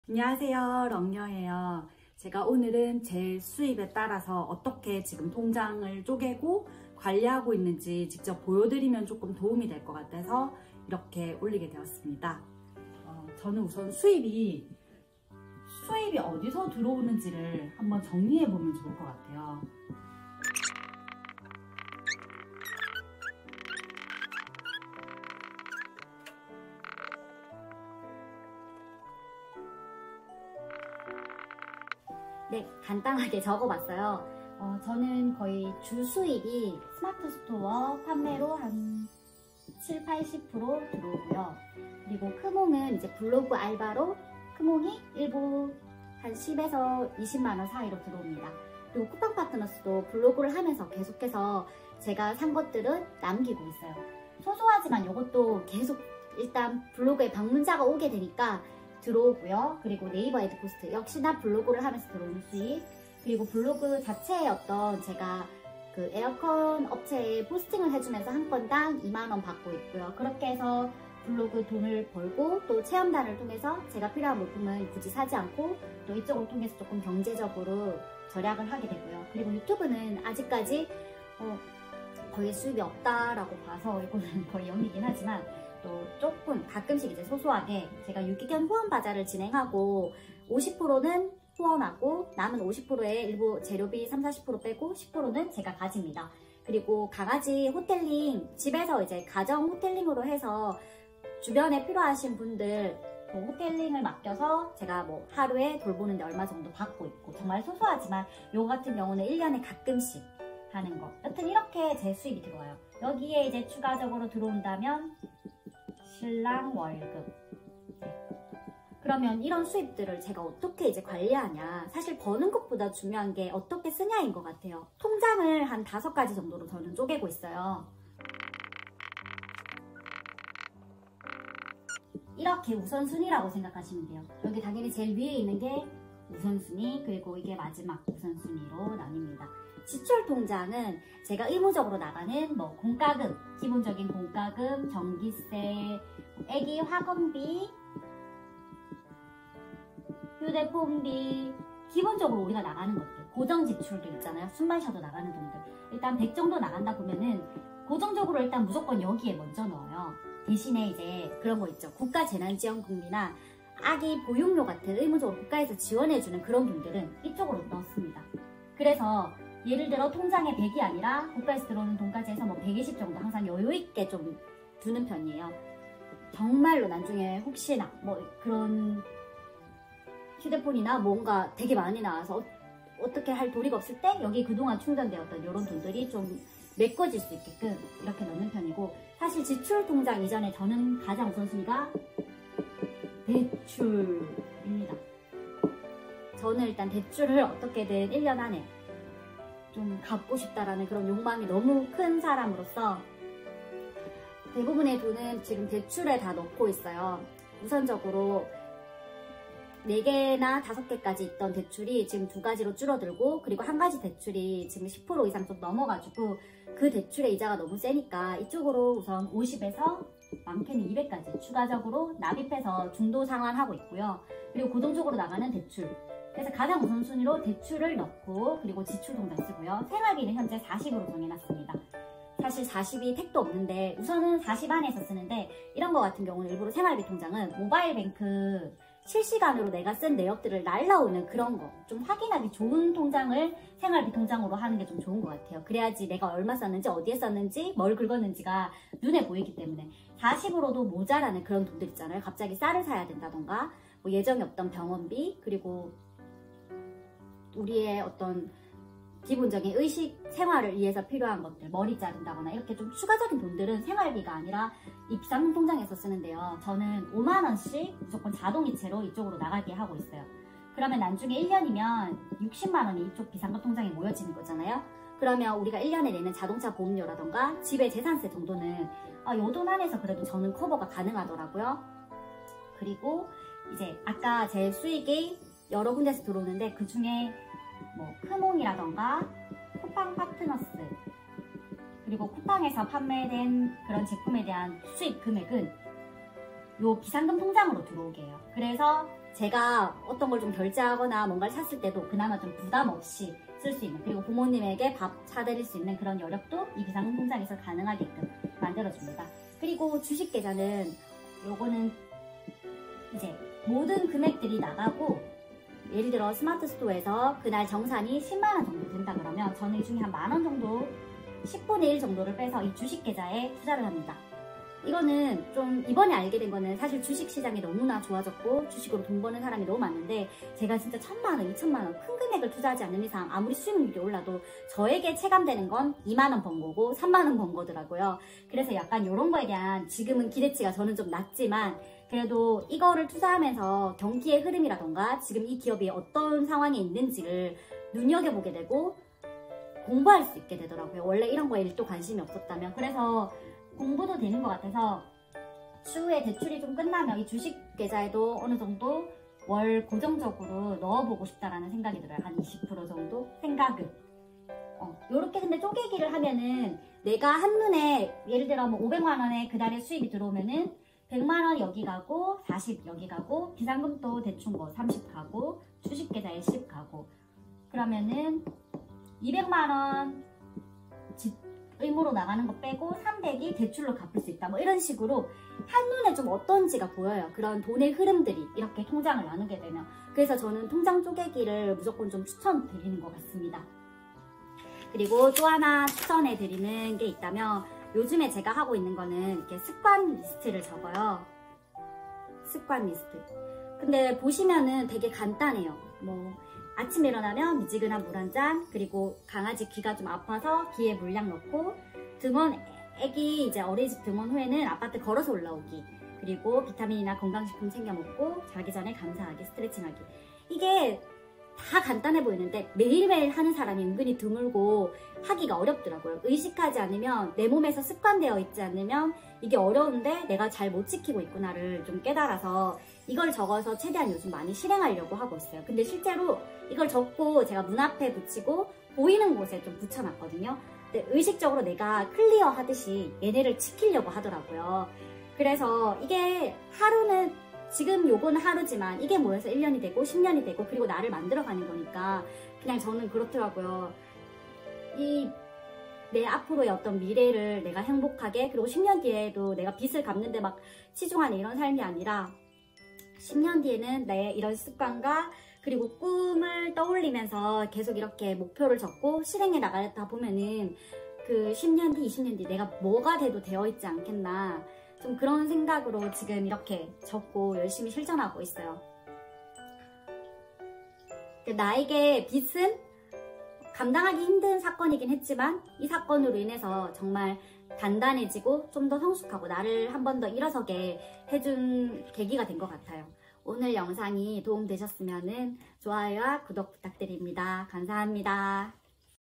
빚갚기프로젝트! 들어오는수입에 따라 통장쪼개기! 마인드셋!
마인드셋 !
버는것보다 어떻게 쓰느냐가 중요하다!
잘 쓰고 싶고 저 역시도
다른이들에게 잘 쓰이고 싶어요.

도움이되는 정보 더 많이 공유할게요!